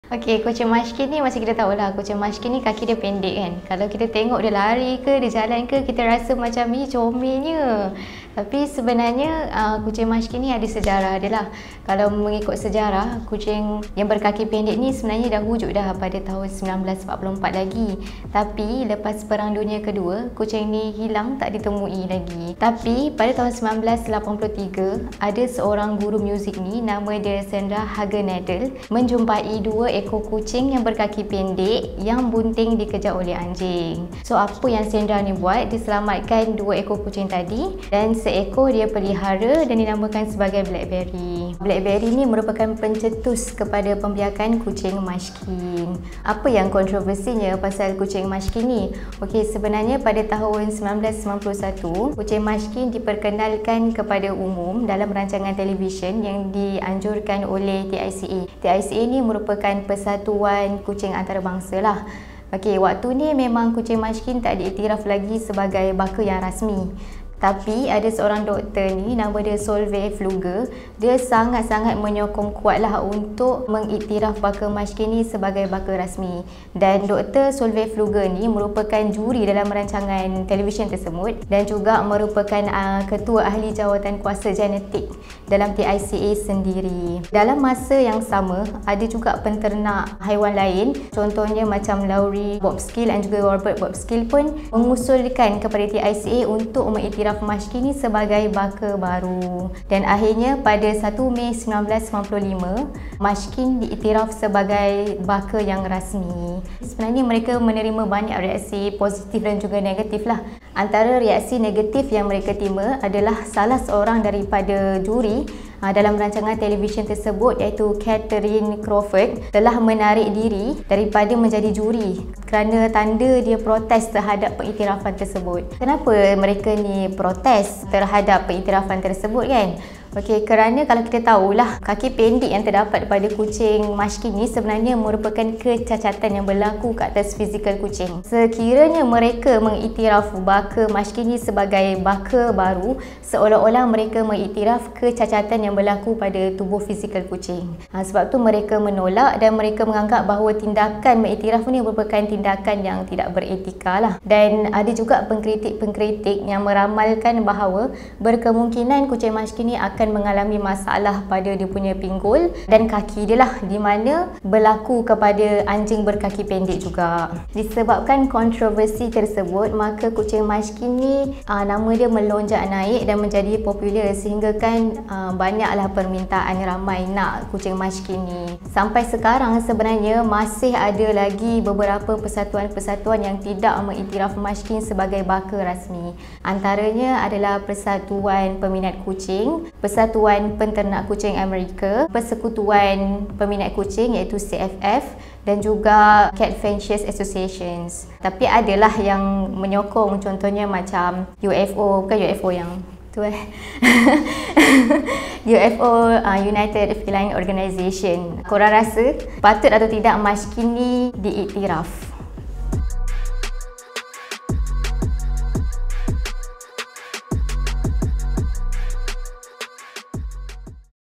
Sejarah dan Kontroversi Munchkin
Rupanya kaki pendek tu dikira sebagai kecacatan. Tapi comel. Lesung pipit pun kecacatan otot kan?